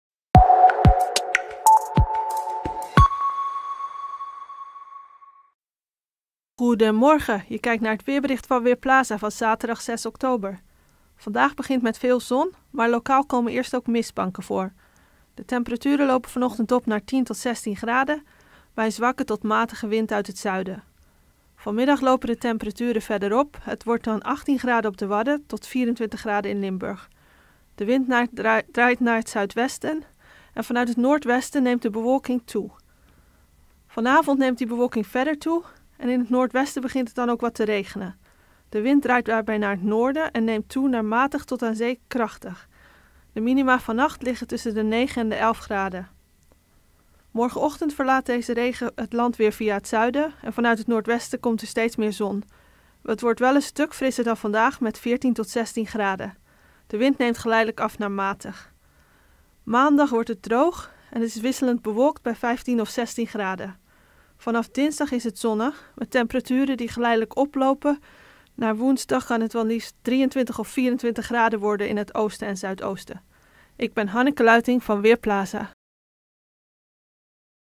Weerbericht zaterdag: vandaag prachtig herfstweer, morgen een stuk frisser
De ochtend start op de meeste plaatsen zonnig, lokaal komt een mistbank voor. In de loop van de dag neemt de bewolking vanuit het westen toe, in het oosten blijft het lang zonnig. Tot de avond blijft het droog en het wordt 18 tot 24 graden. In de avond en nacht naar zondag valt er soms wat regen. Morgen trekt de regen of een enkele bui zuidwaarts weg en klaart het op. Met 15 of 16 graden is het wel koeler. Maandag is het wisselend bewolkt en droog bij maximaal 18 graden. Dinsdag is het op de meeste plaatsen zonnig en warm bij maximaal 22 graden.